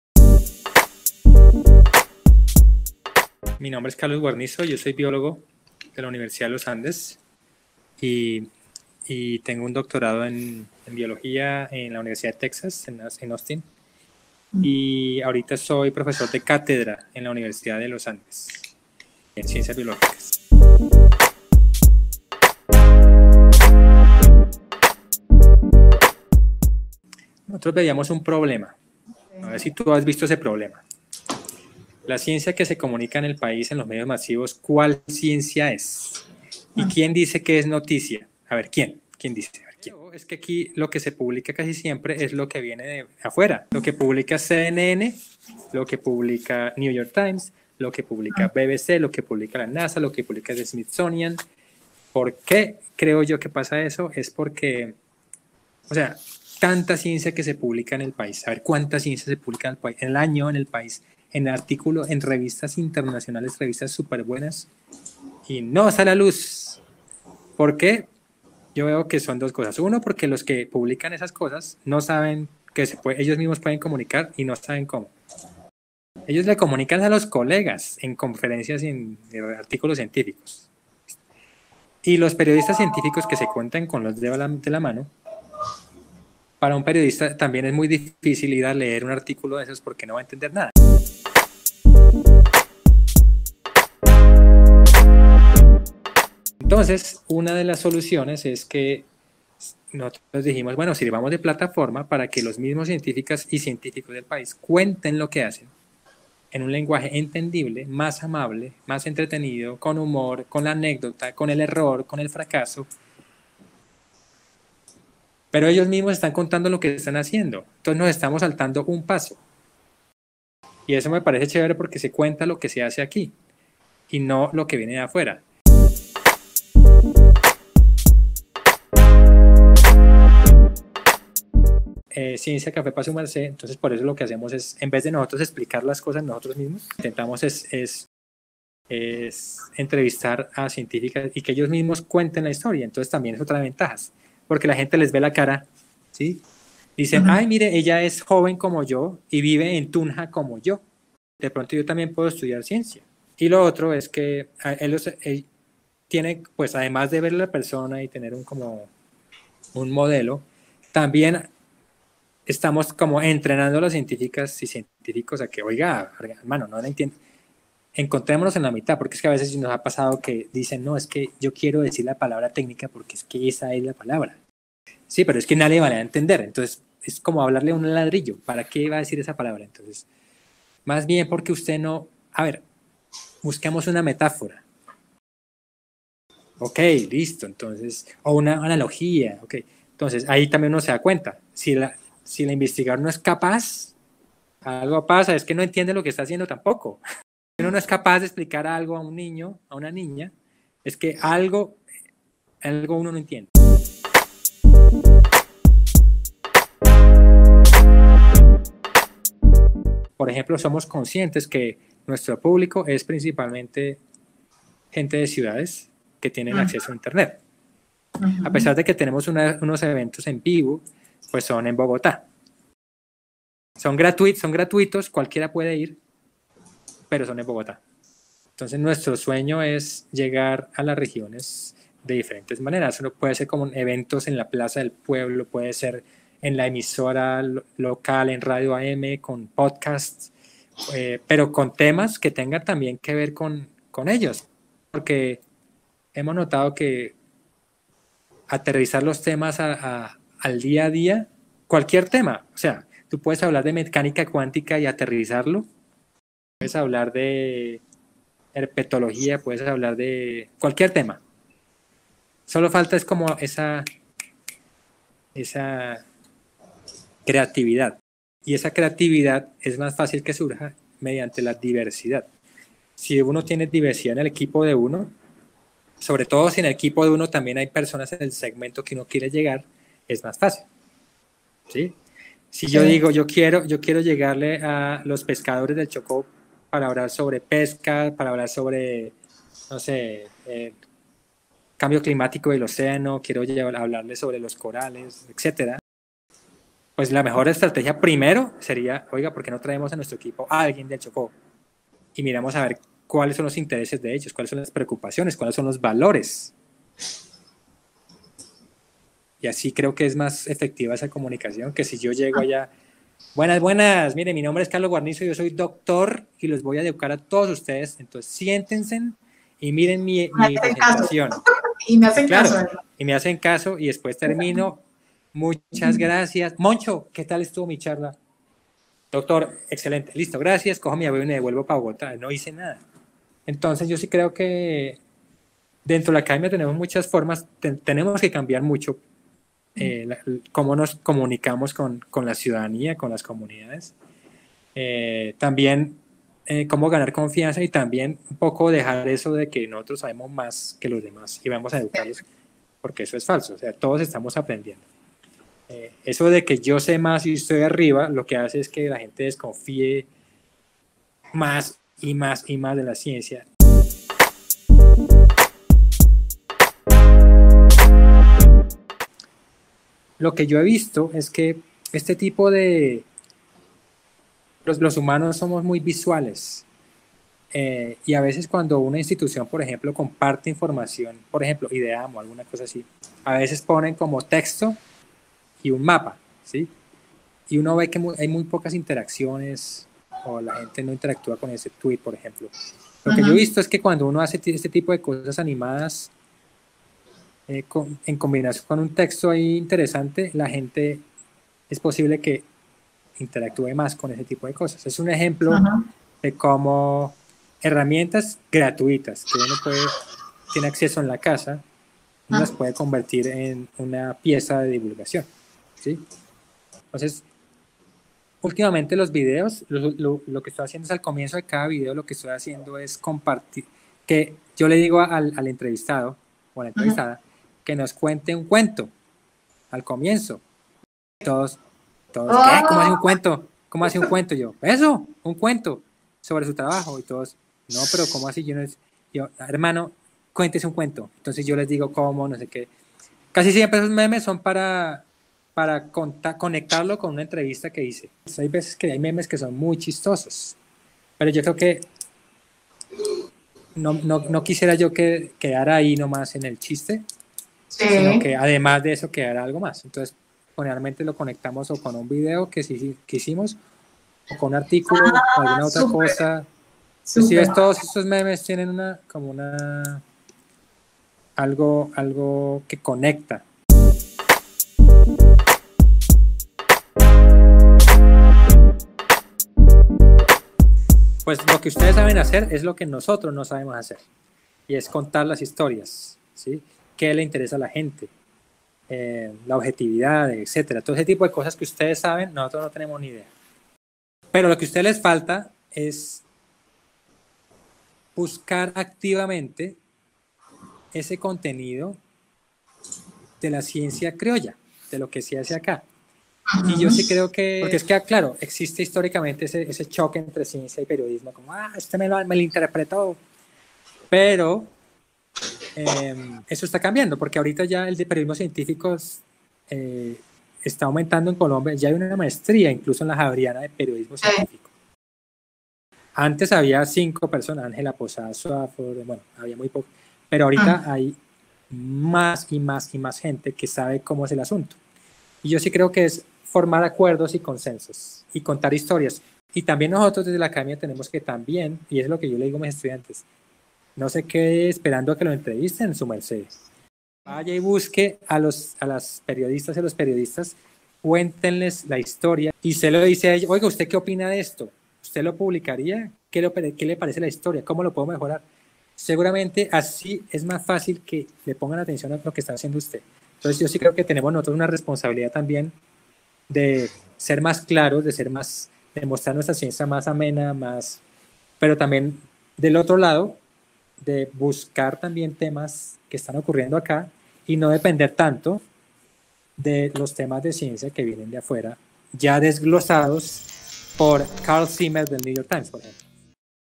Comunicar la ciencia
Entrevista realizada a Carlos Guarnizo, doctor en Biología, fundador y líder de la iniciativa 'Ciencia café pa' sumercé', en la que nos explica el valor y la oportunidad que trae generar estrategias y contenidos basados en investigación científica para comunicarse con audiencias amplias, desde perspectivas educativas e incluyentes.